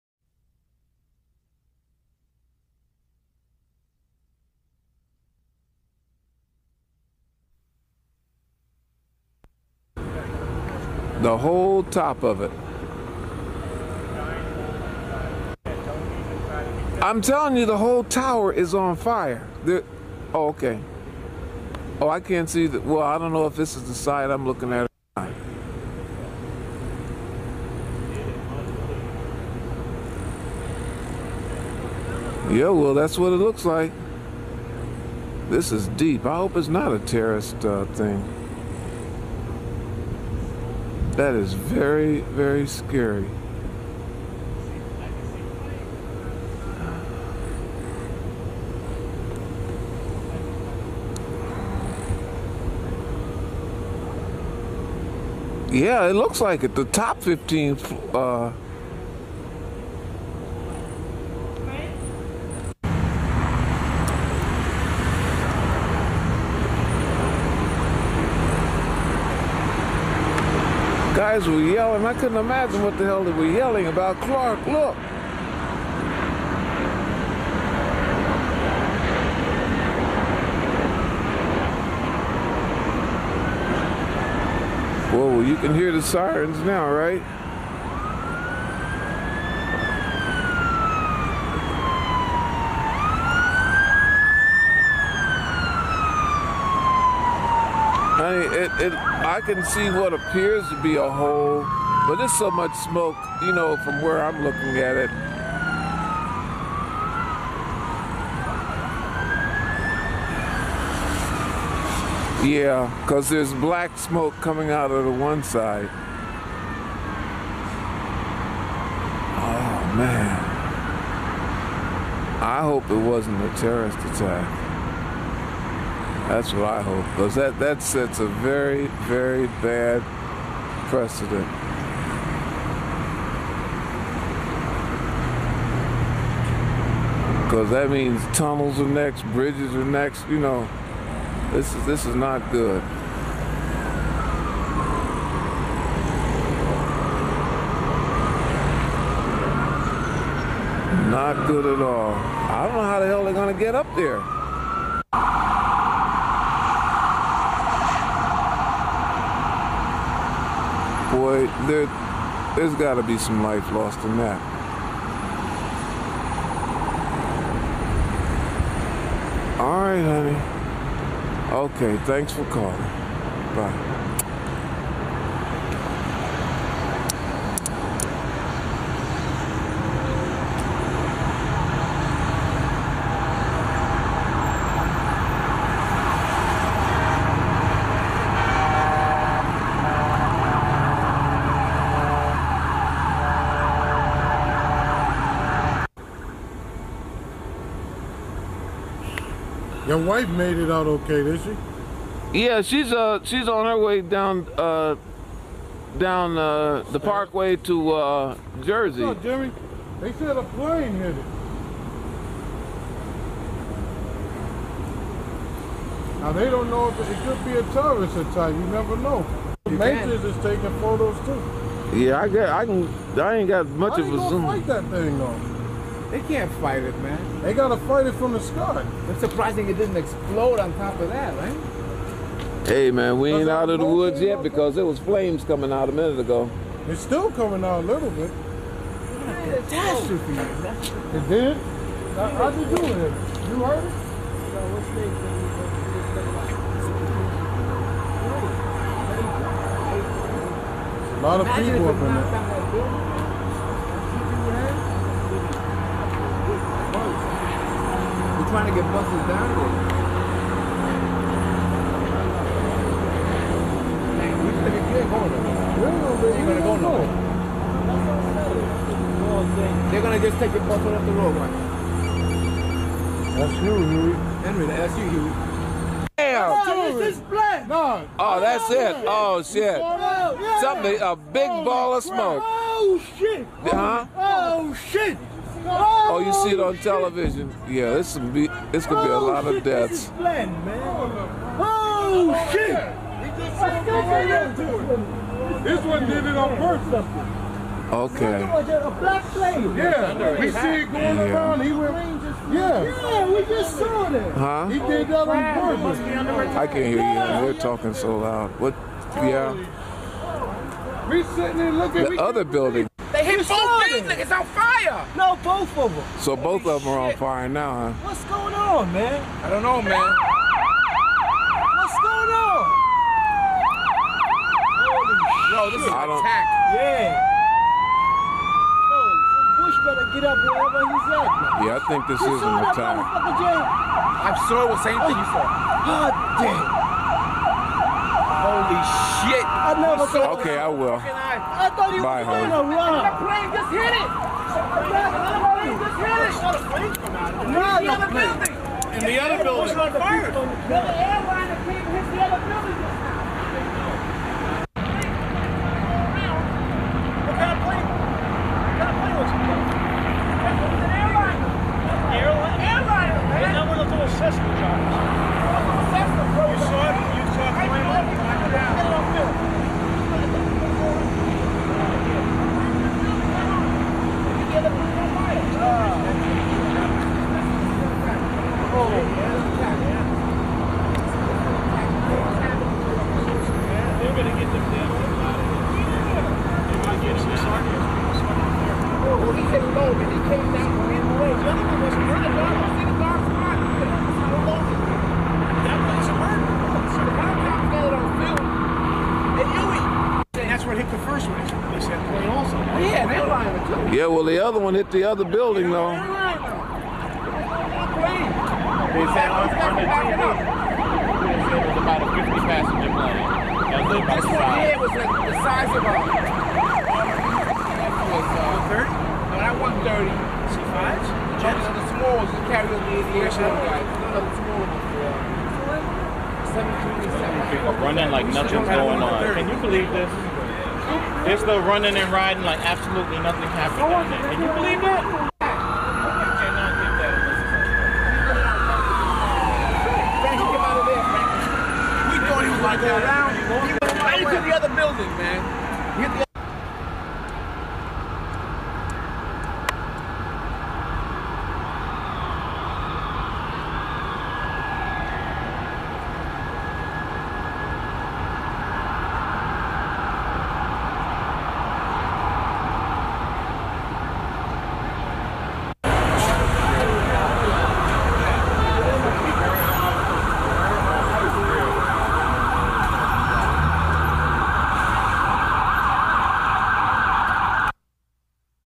9/11 Footage (WTC, Hubert Clark)
0:10 : The video starts

4:36 : Approximately two seconds after United Airlines Flight 175 impacts the South Tower of the World Trade Center.